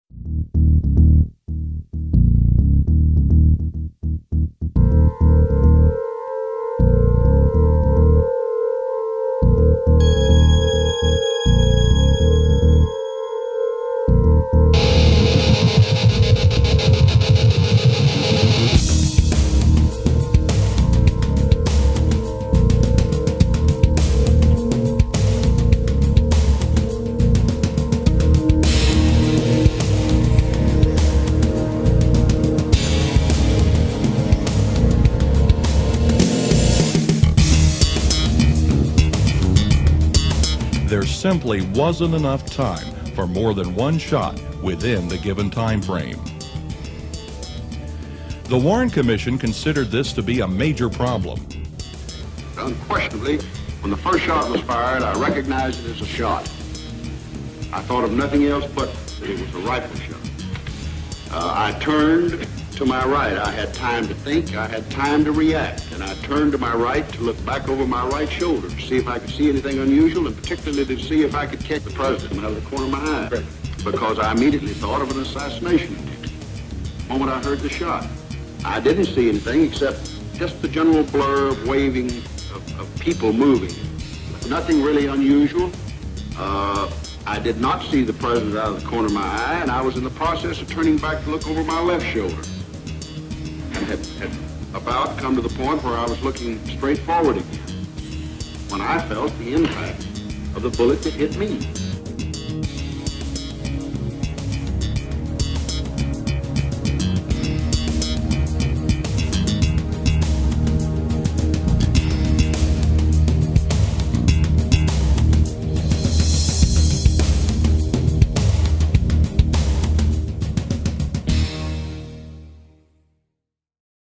JFK and The "Magic Bullet" Theory
This is a short film I made to debunk the magic bullet theory.